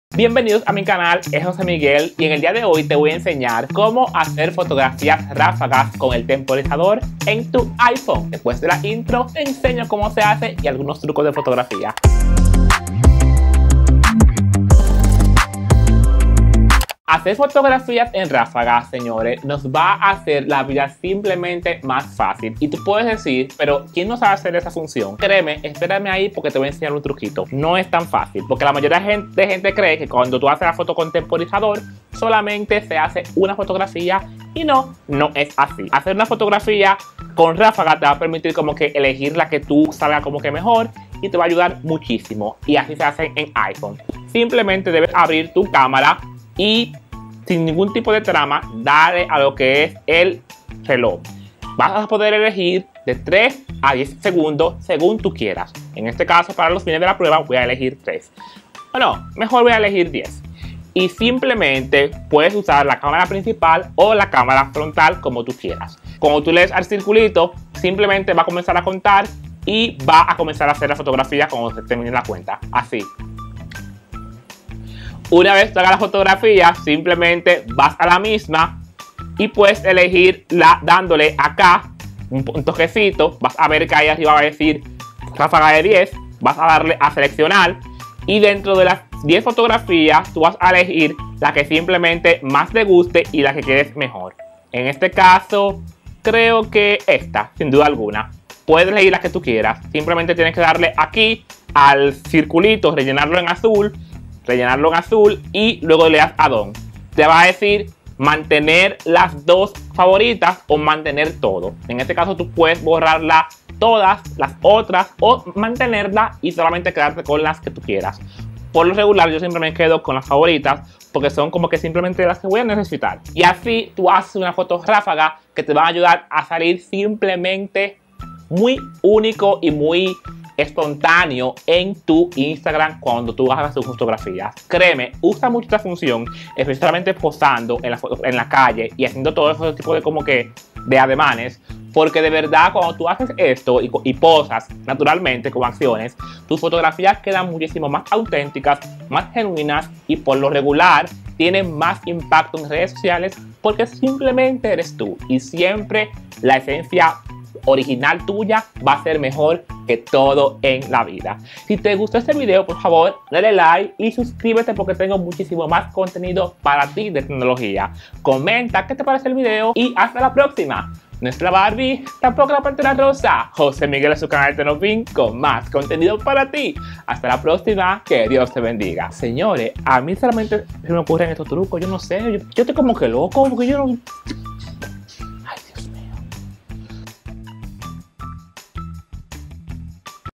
TRUCO Cámara iPhone | Como Hacer Ráfaga en iPhone con Temporizador
Este video se trata de Como Hacer Ráfaga en iPhone con Temporizador o  Como tomar varias fotos con temporizador en iphone un truco con la camara del iPhone.

📷 EQUIPO:
⇢ Cámara : Canon t7i.
⇢ Programa para editar: Final Cut Pro.
⇢ Computador: MacbookPro 2017.